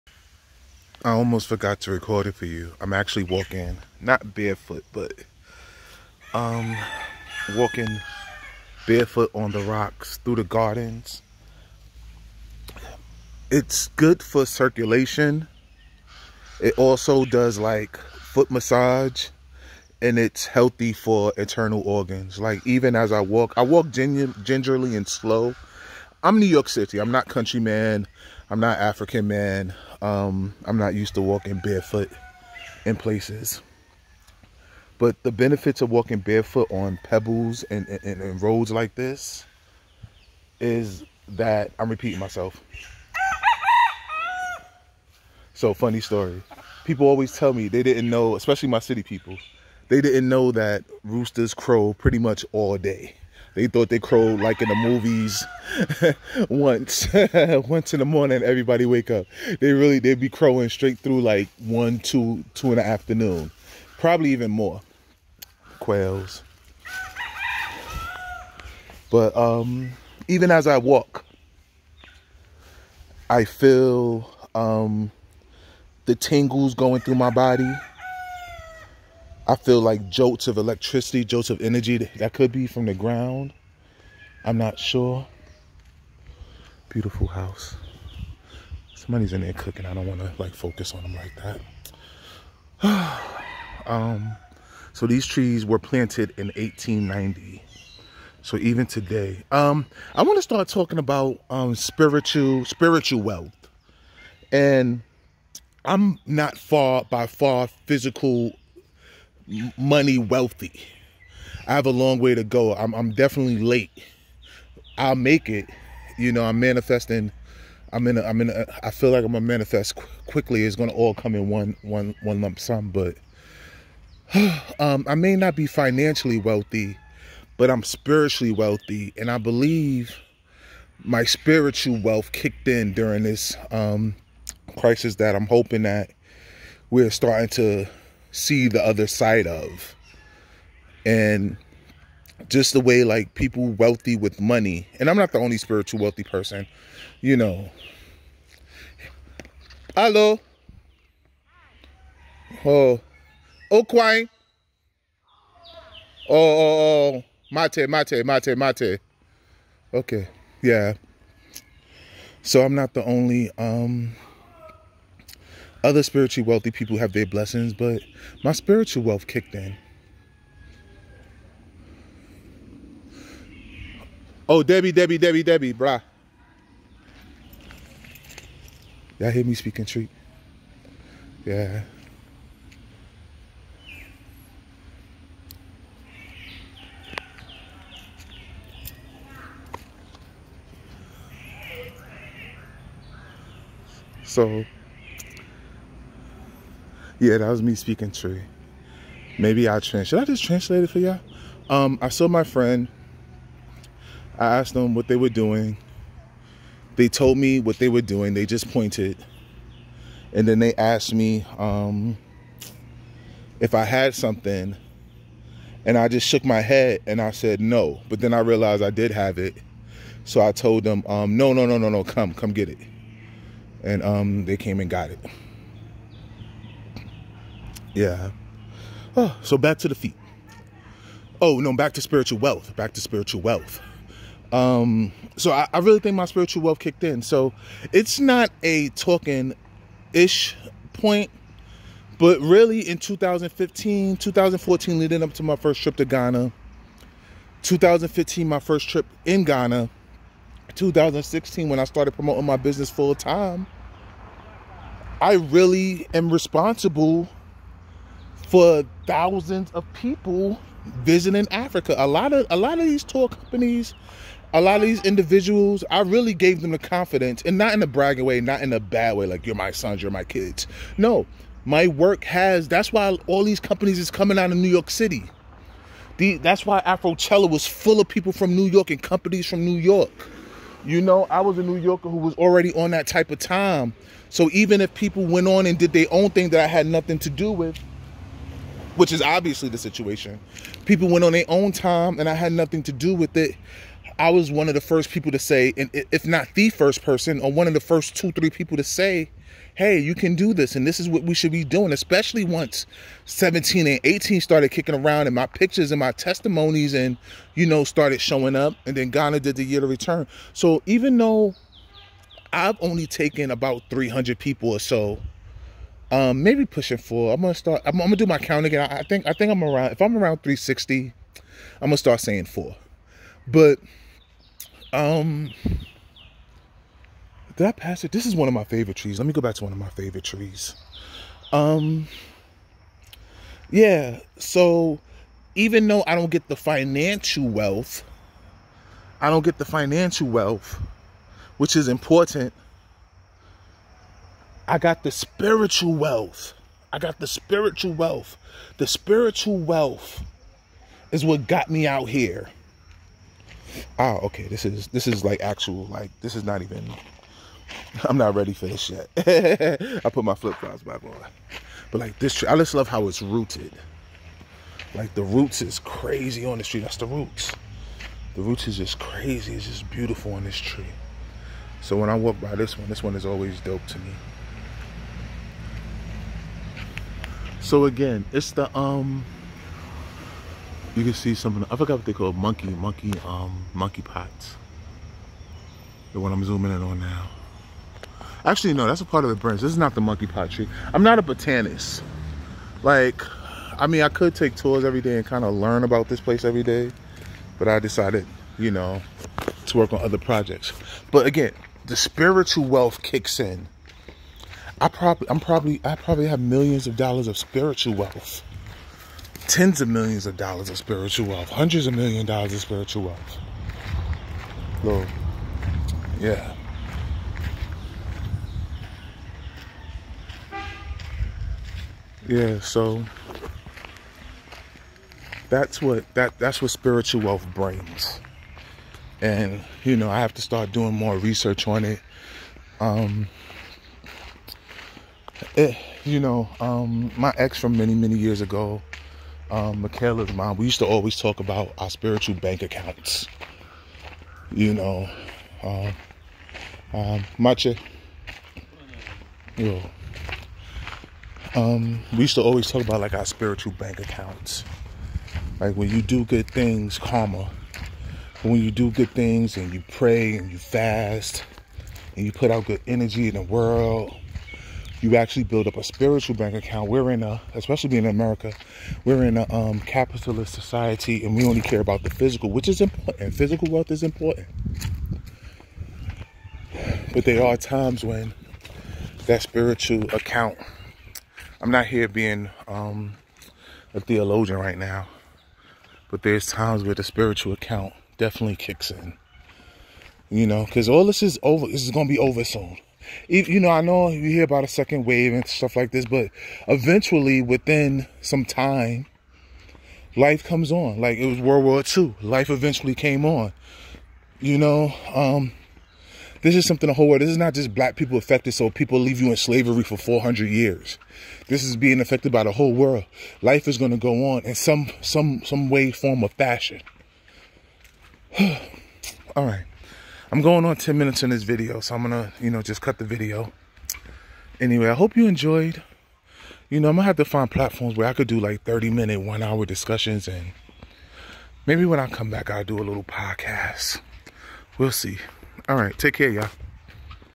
Bare foot walk through Aburi Gardens
Rashad McCrorey discusses spiritual wealth,  blessings, and healing as he walks through Aburi Botanical Gardens